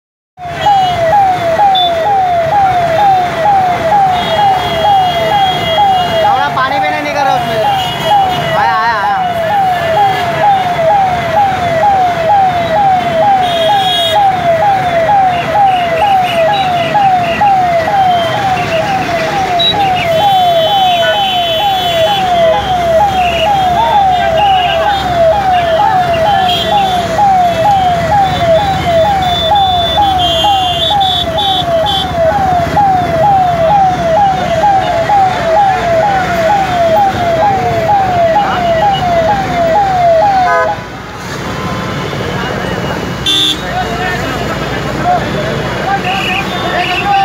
Car Burning In Mumbai on Busy Western Express Highway LIVE FOOTAGE
Car Burning In Mumbai on Busy Western Express Highway

LIVE FOOTAGE

Fireman doused fire of the care

No injured and dead reported